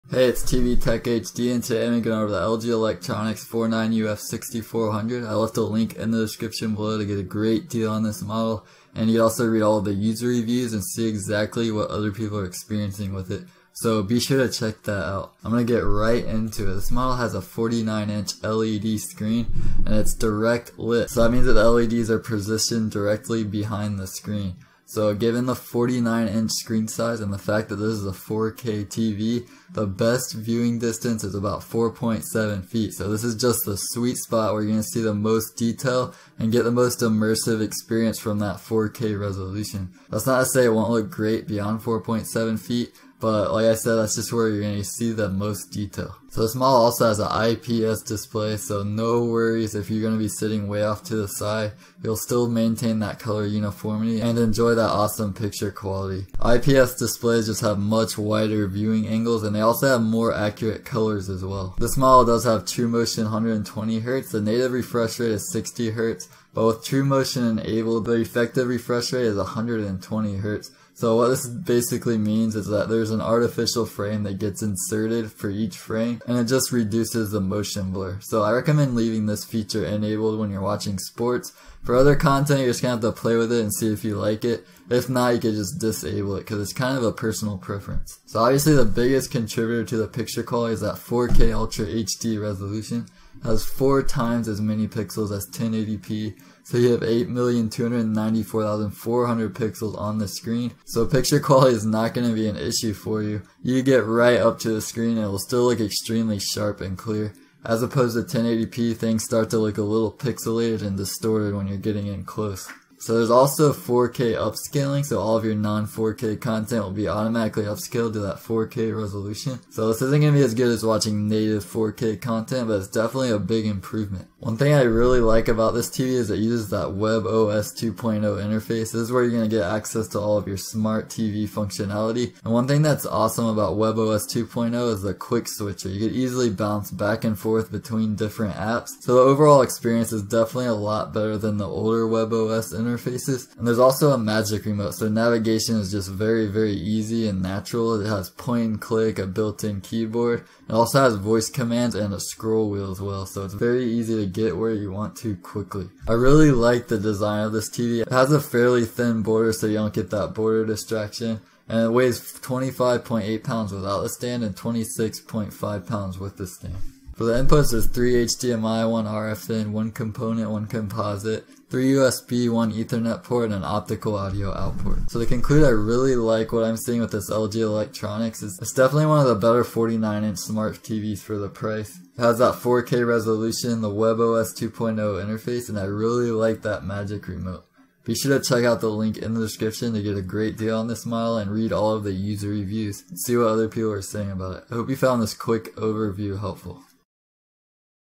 LG Electronics 49UF6400 49" 4K Ultra HD LED TV Review
LG Electronics 49UF6400

This is my review of the LG Electronics 49UF6400.

I break down what all of these confusing specifications and features means so you can make a more informed and confident decision.

Refresh Rate: 120Hz (Effective)
Backlight: LED (Direct LED)
Smart Functionality: Yes